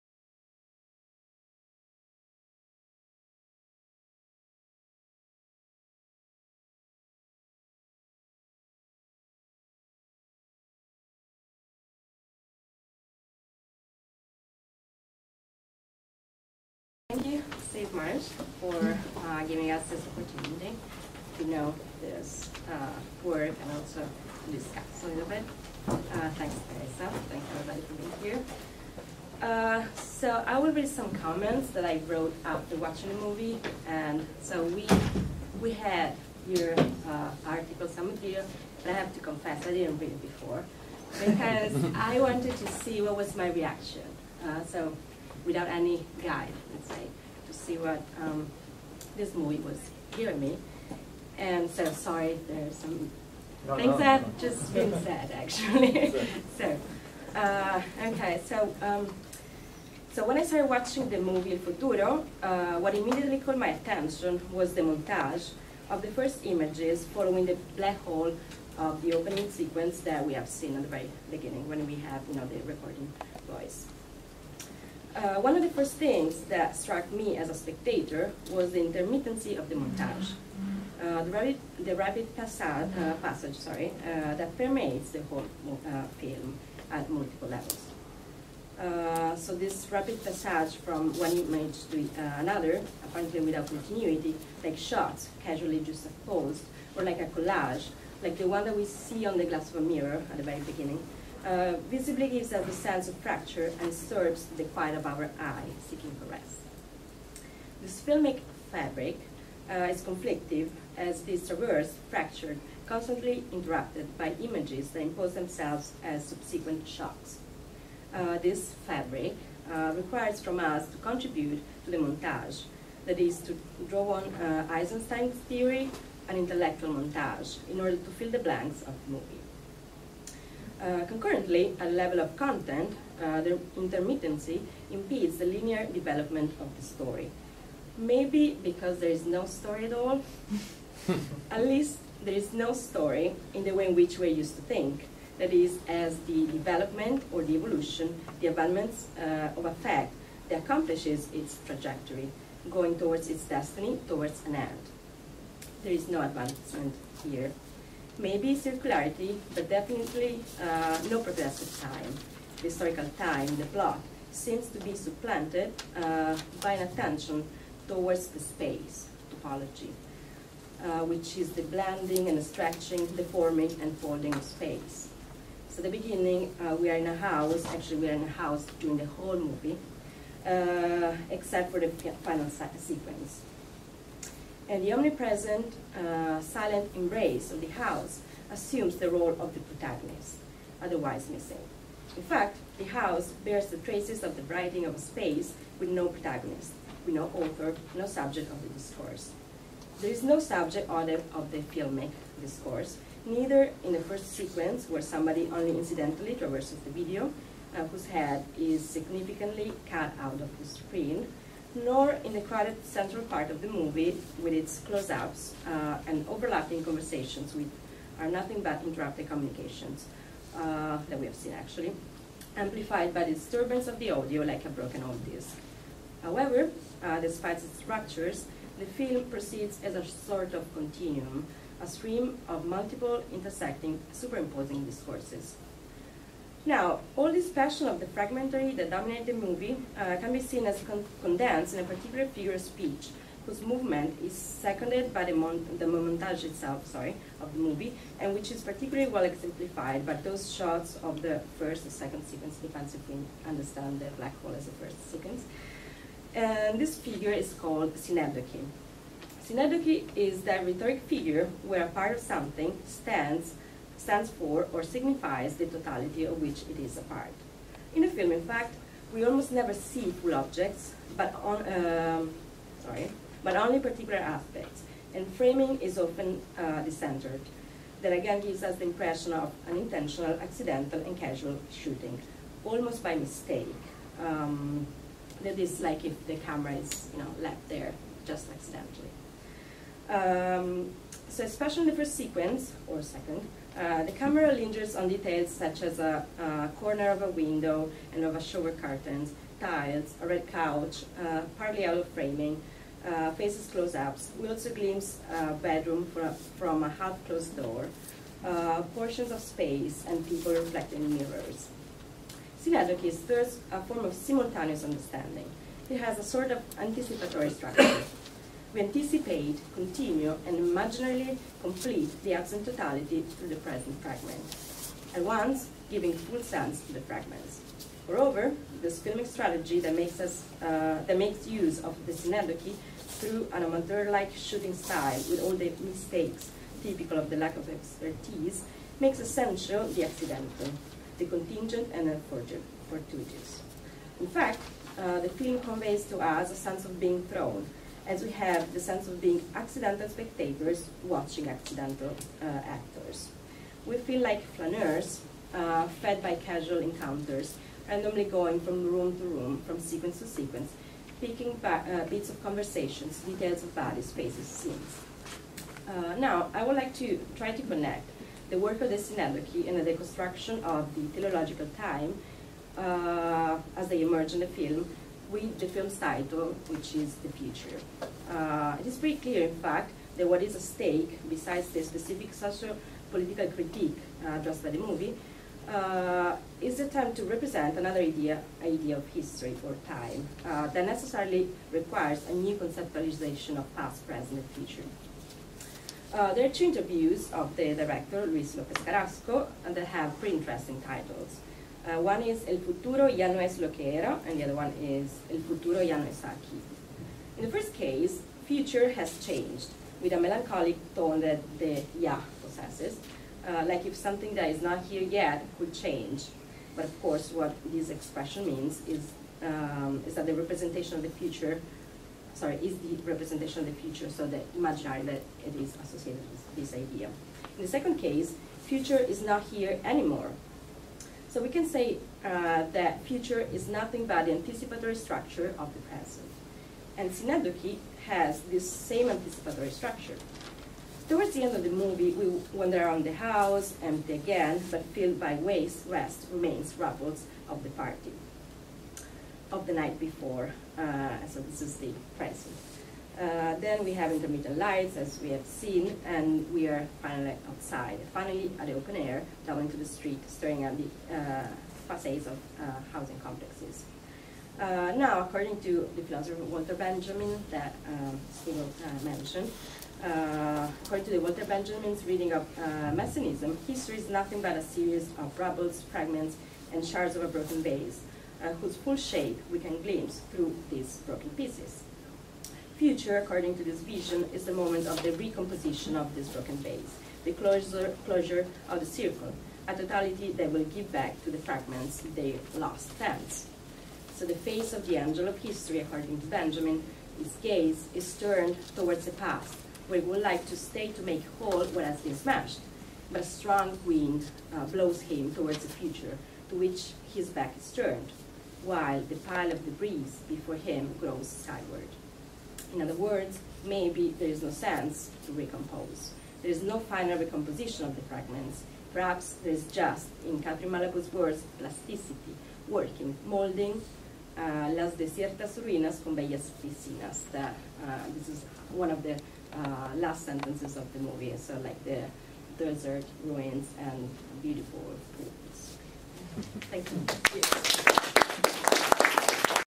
5. Cinema / Activism / Thought - Michela Russo: Panel on prof. Marsh's lecture
Cinema / Activism / Thought

Texas A&M University.
College Station, Texas.

Prof. Steven Marsh (University of Illinois at Chicago) - "Retrospective Future Perfect: Blackholes and Time Tunnels in Luis López Carrasco's 'El Futuro'"

Panel on prof. Marsh's lecture. Participants:
Prof. Dan Humphrey, Andy Lantz, Rubria Rocha, Michela Russo

Prof. Tatjana Gajic (University of Illinois at Chicago) - "Tragic Thresholds: Rafael Dieste and María Zambrano's Philosophical Poetics".

Organized by prof. Teresa Vilarós-Soler.